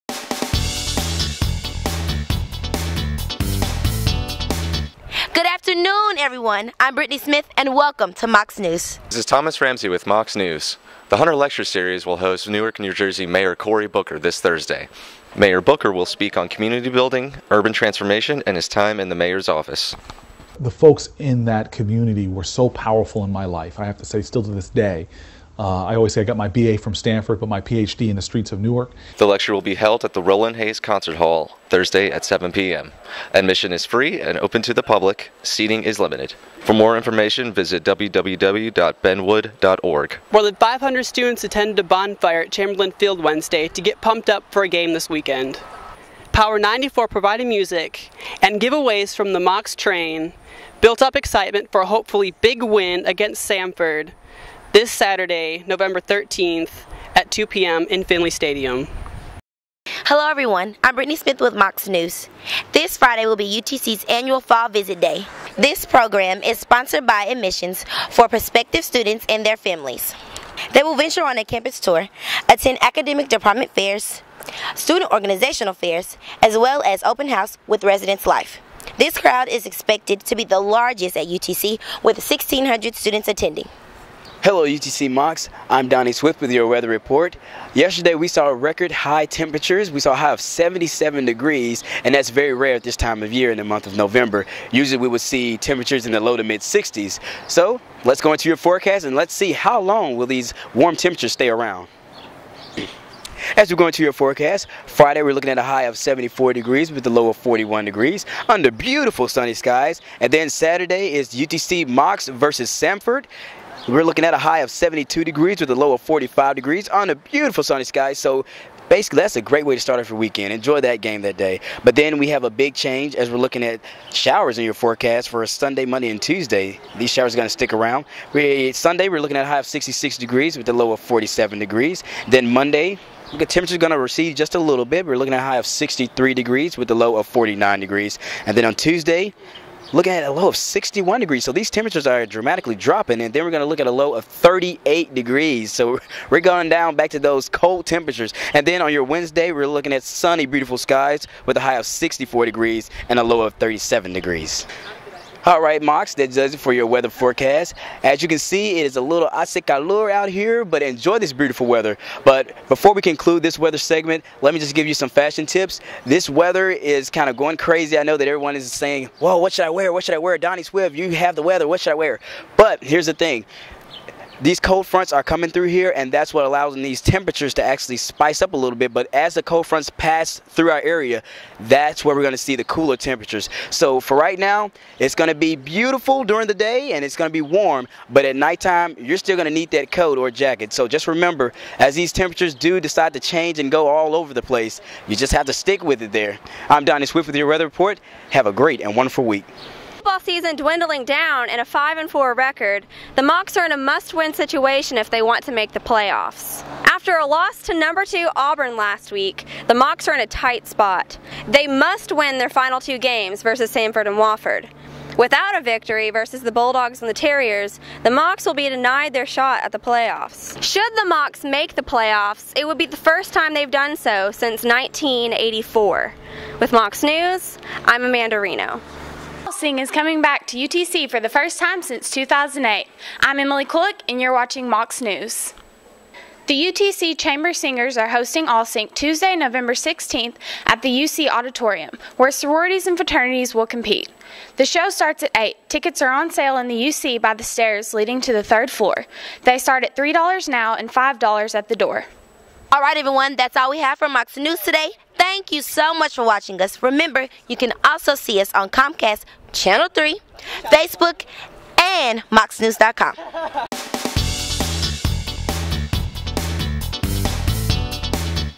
Mocs News 11-11
The mayor of Newark, Cory Booker is coming to UTC. Also find out about All-Sing and the must win situation for the UTC Mocs football team. See the bonfire leading up to the Samford game and learn about Fall Day on campus.